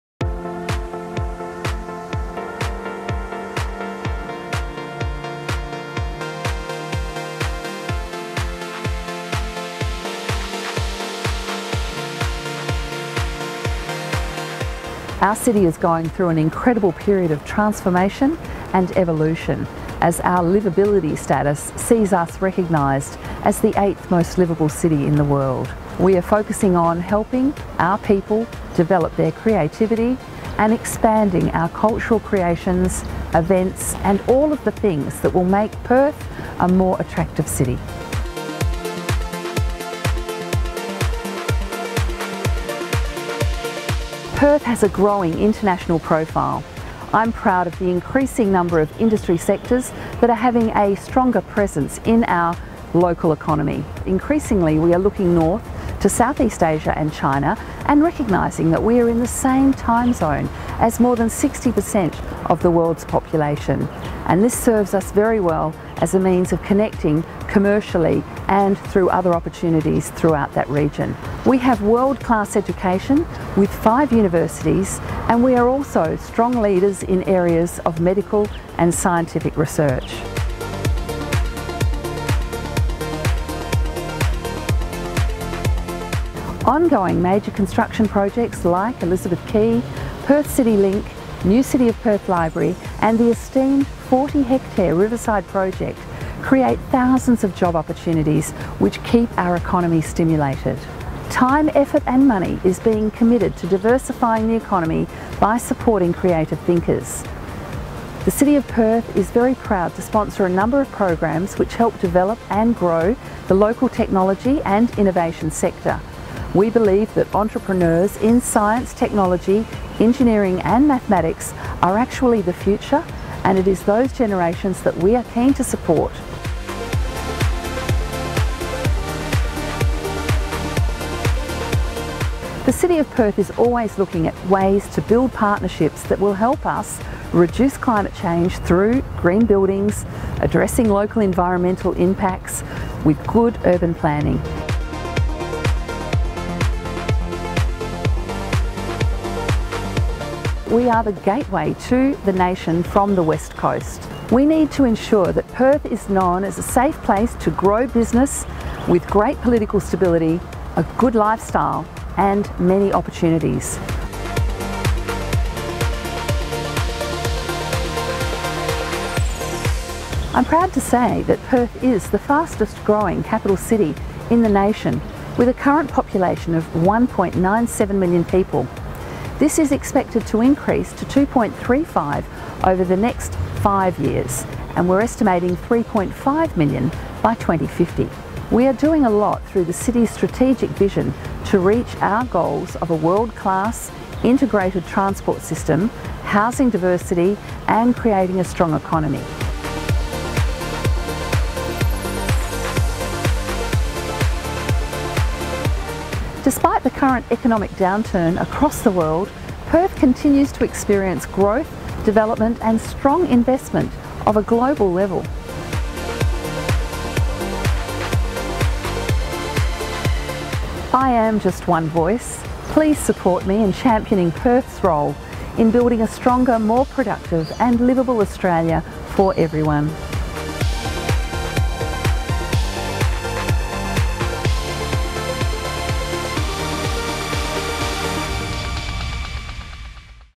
Lord Mayor Lisa Scaffidi on how Perth is helping to support the nation
Australia’s Council of Capital City Lord Mayors is championing the shared interests of our great cities to create a stronger, more productive future for us all.

The Cities Matter campaign aims to engage residents and workers to help increase our share of voice in shaping national policy at this critical time.